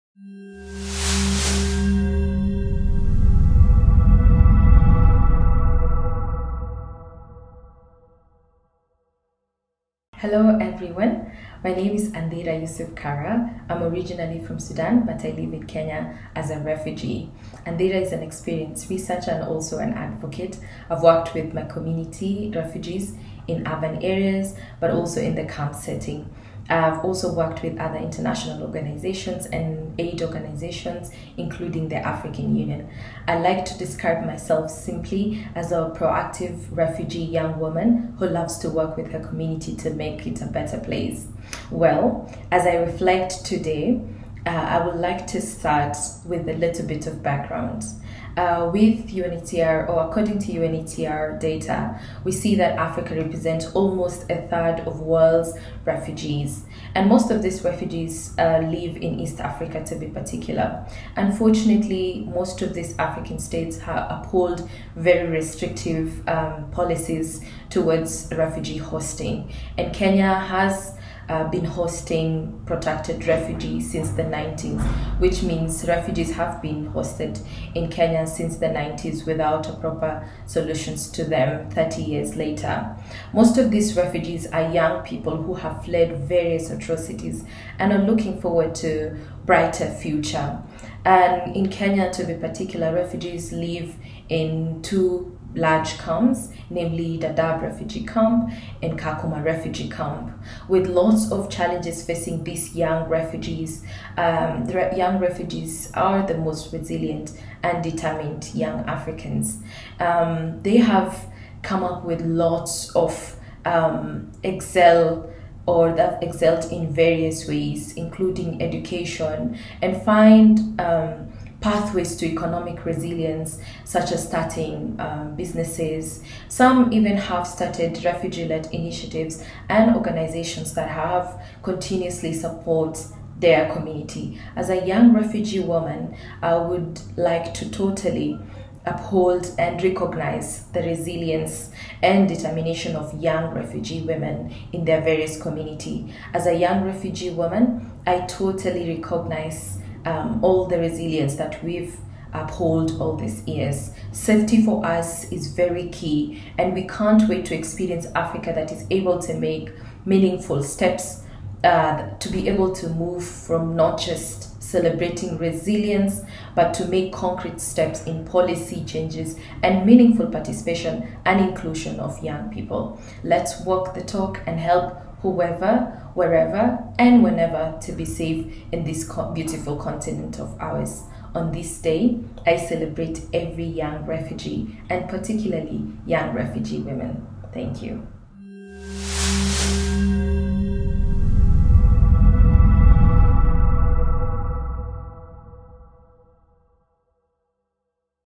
MS. ANDHIRA KARA, A FORMER REFUGEE SPEAKS ON WORLD REFUGEE DAY 2022
As part of AASU's worldrefugeeday commemoration, an interview with Ms. Andhira Kara, Lead Country Researcher - Kenya, Local Engagement Refugee Research Network, throws more light on the plights of refugees.

As a refugee herself, she calls on African governments, organizations and individuals to move from just celebrating resilience to making changes in policies to ensure the inclusion of refugees in the continent.